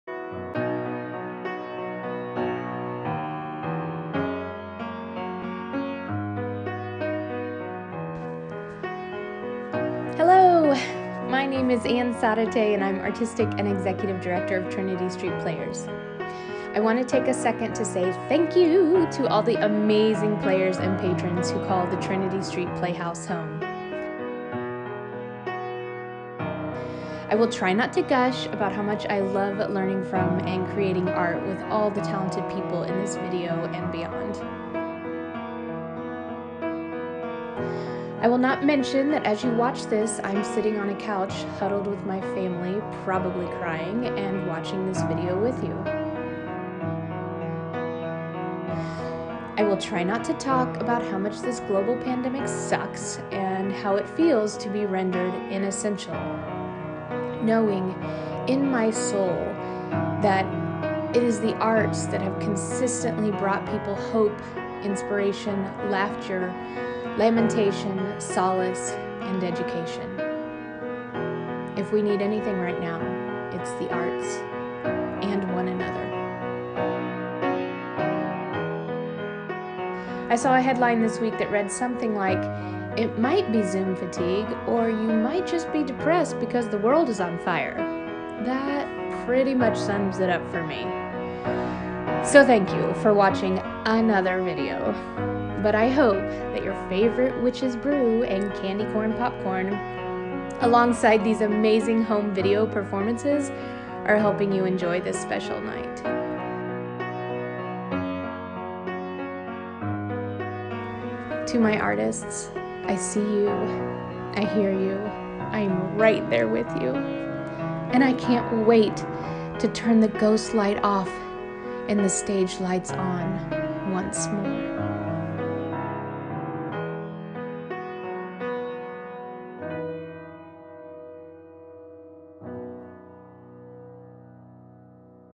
For Our Players... We give thanks!
A message from Artistic Director, Ann Zarate, to our Trinity Street Players. Originally recorded for TSP's Halloween Cabaret I AM THE MASK YOU WEAR, we'd like to give a shoutout to our peeps this season of thanksgiving. We'll be together again soon!